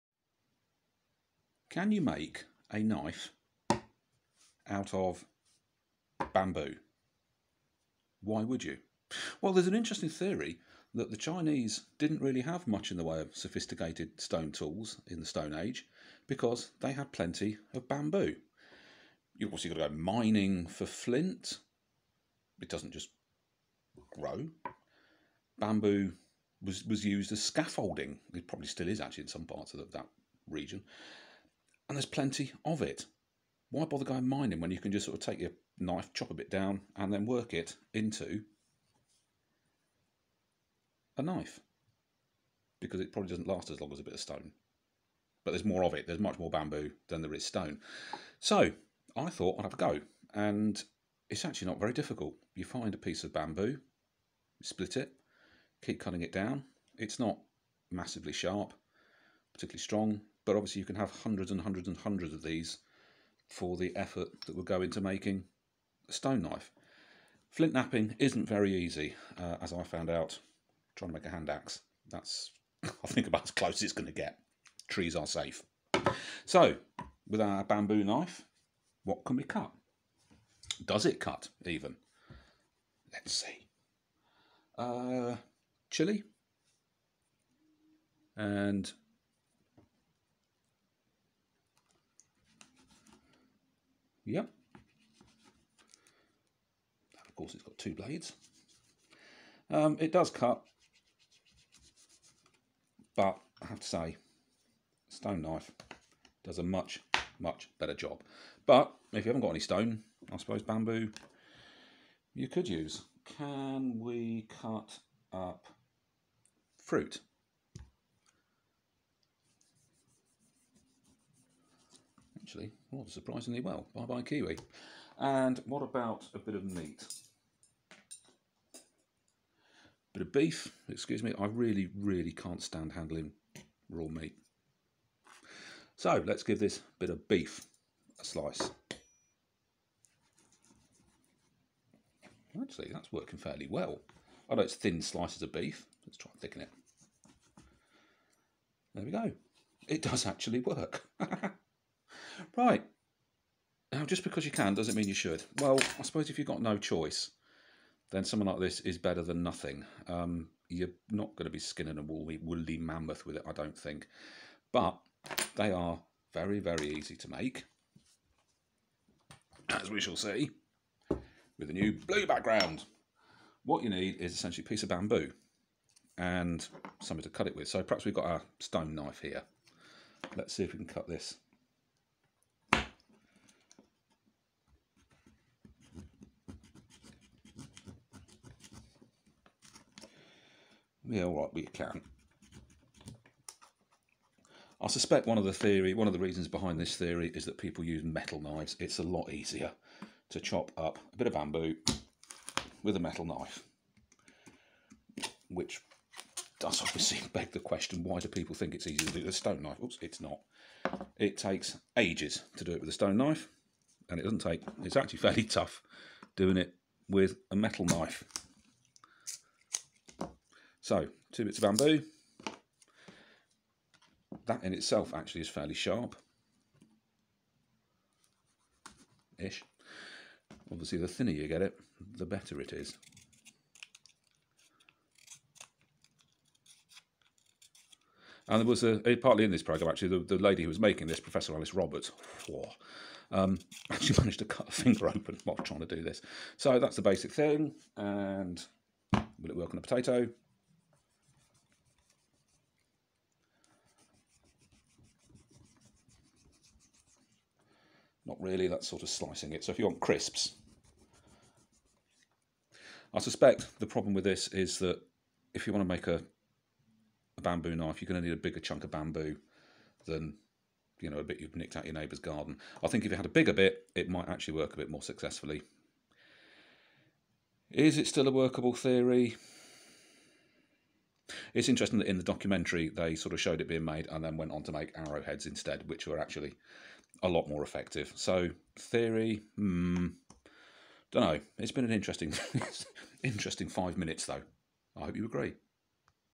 Bamboo knife, as used in the Chinese stone age....maybe.
Experimental archeology!
I`m going to do more research and take this further having found a supplier of bamboo off-cuts. More later.